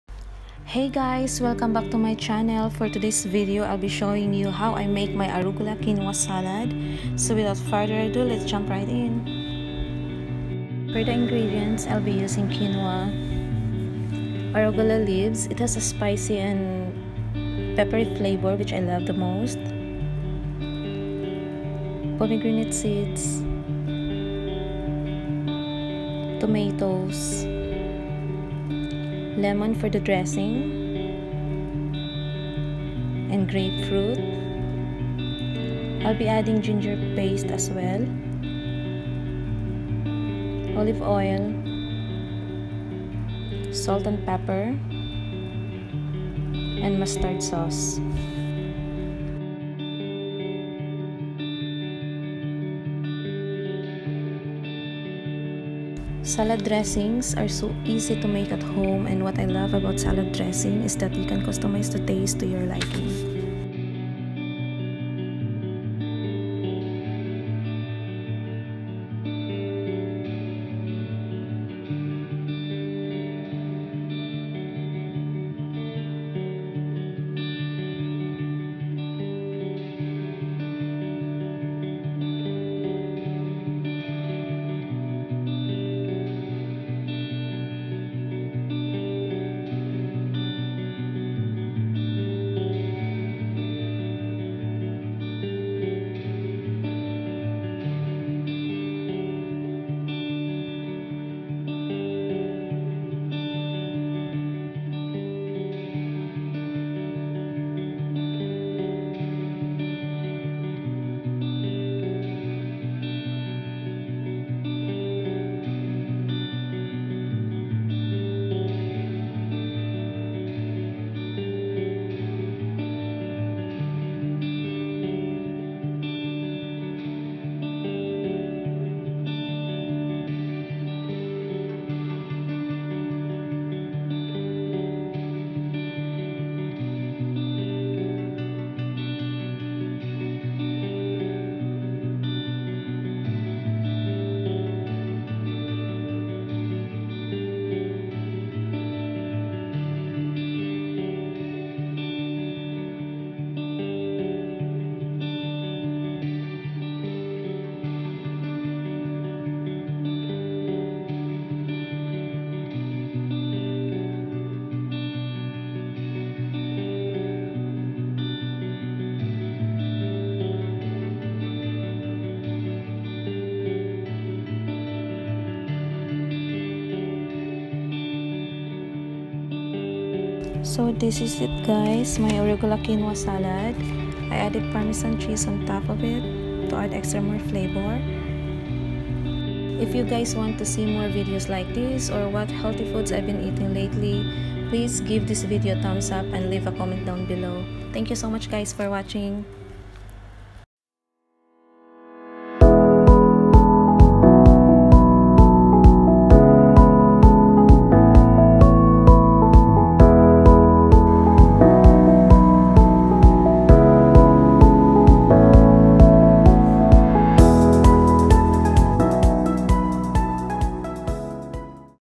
Arugula Quinoa Salad
These easy and healthy salad is good for lunches and dinner,actually this is one of my go to salad its super healthy,quick and easy to make and so delicious.enjoy